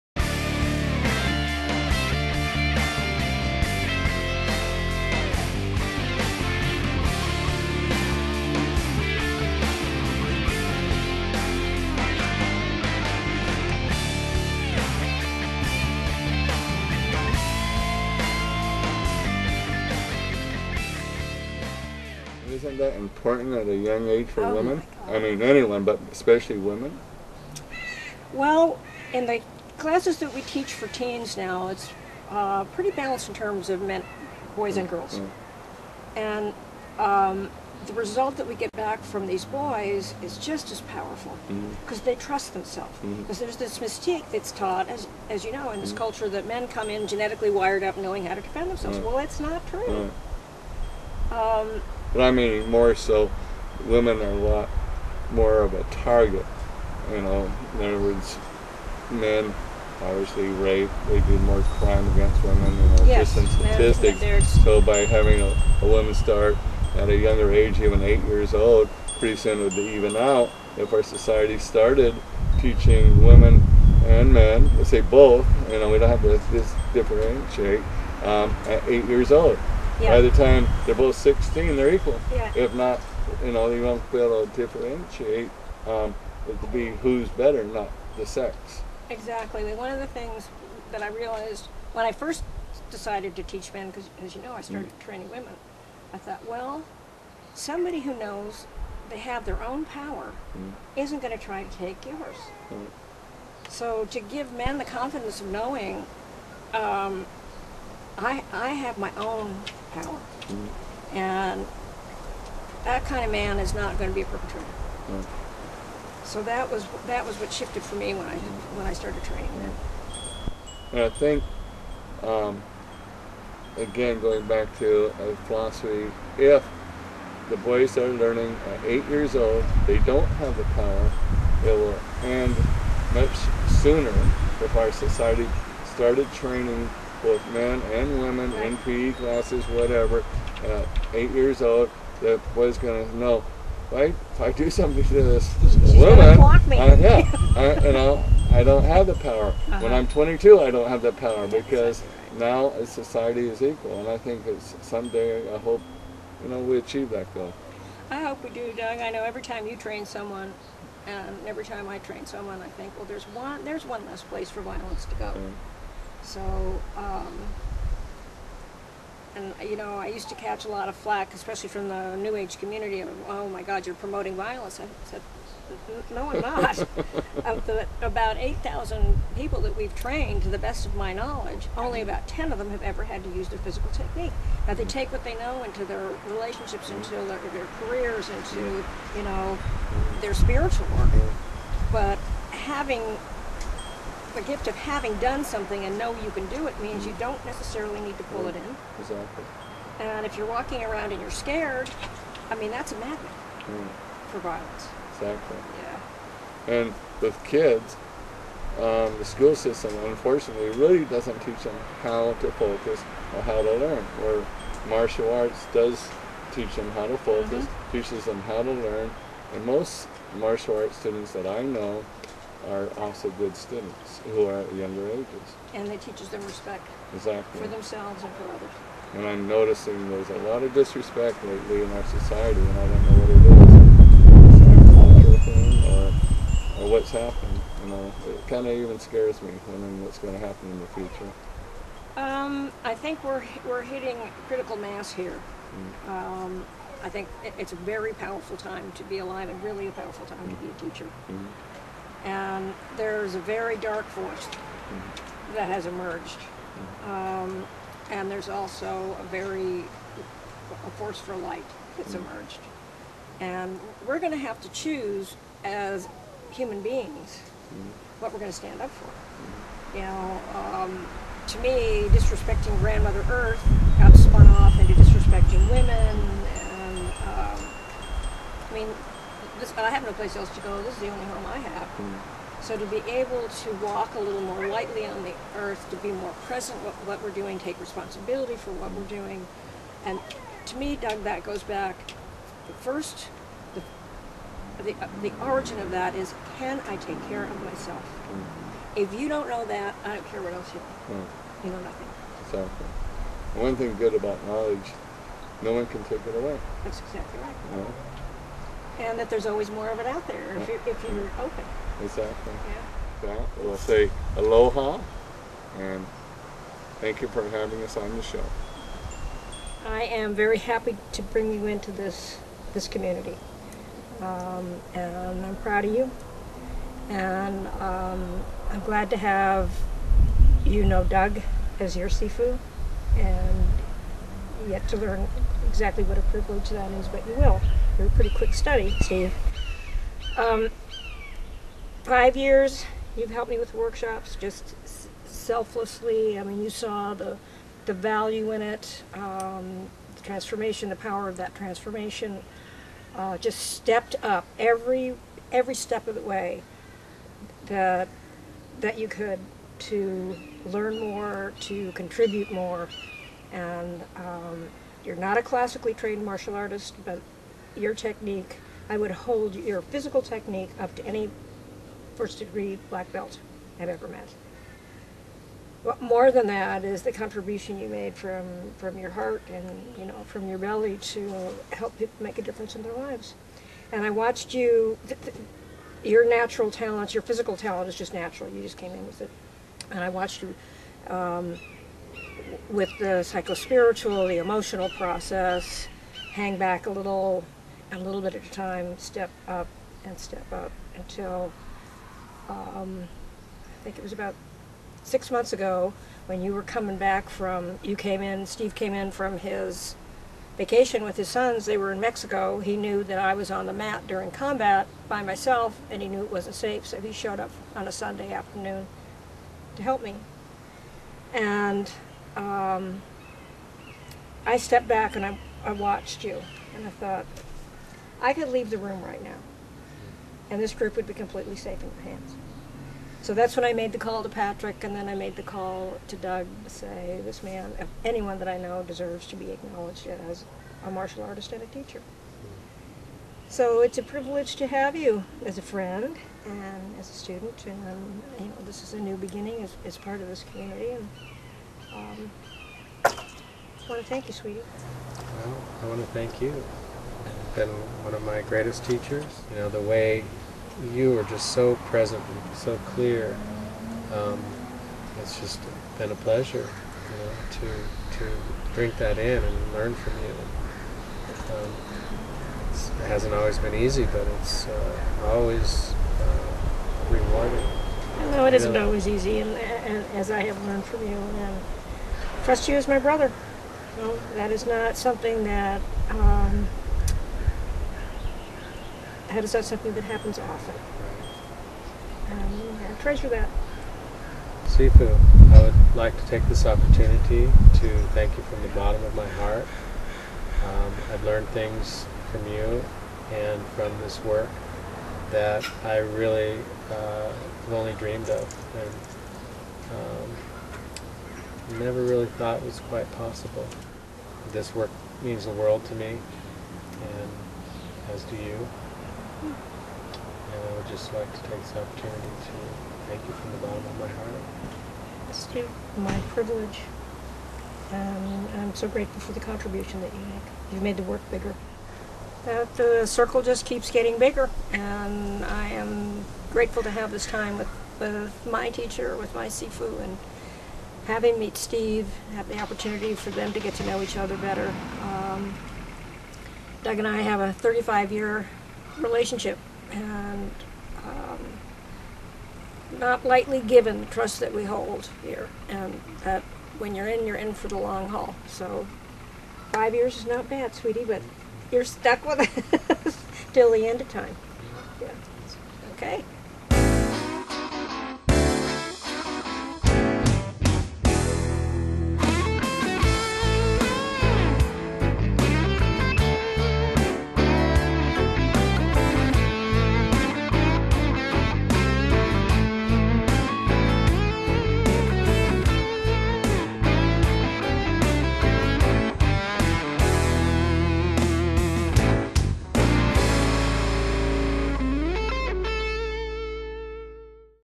Famous Martial Arts Teachers3
Part three of conversation between Grandmaster Douglas Mcleod, and student Grandmaster Dawn Callan. Dawn Callan honors promoted student Steve Milne(1st Degree Black). 2007